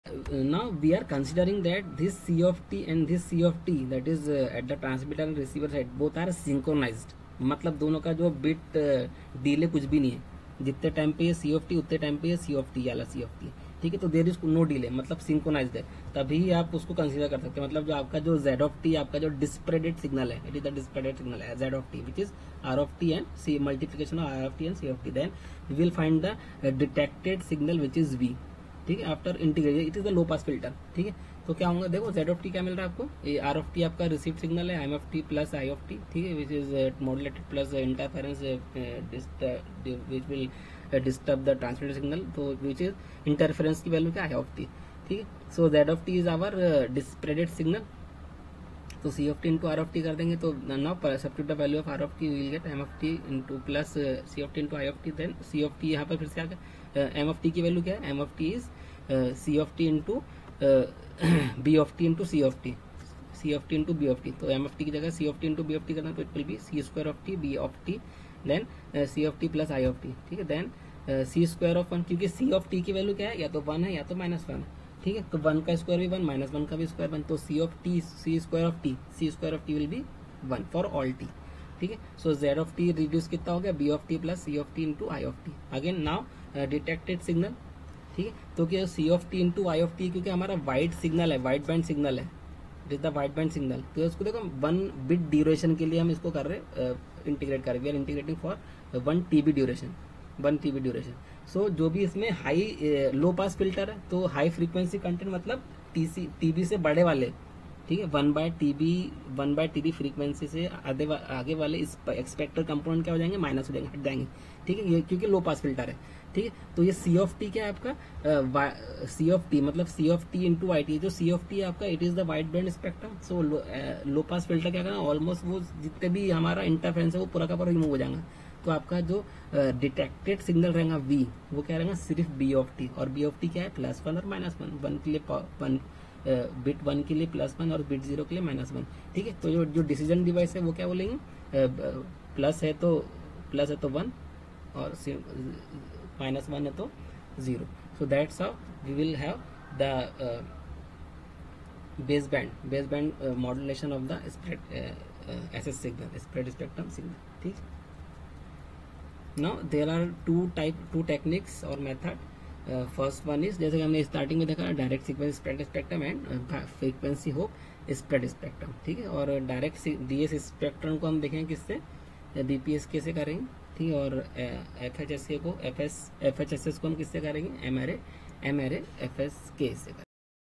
Digital Communication lecture 6.3.2
Baseband model of spread spectrum systems
Tb, Tc, b(t), c(t), r(t), z(t), v